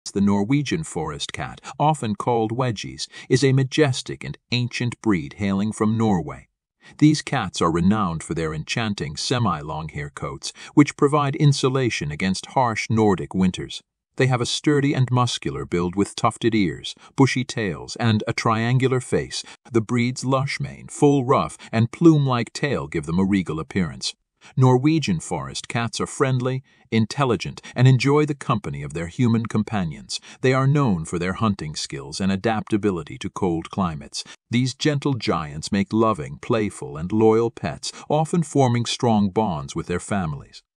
Norwegian Forest cat
These cats are known for their large and sturdy build. They are considered one of the largest domestic cat breeds, with males typically weighing between 13 to 22 pounds (6 to 10 kilograms) and females between 8 to 16 pounds (4 to 7 kilograms). They have muscular bodies and strong legs, making them well-suited for climbing and outdoor activities.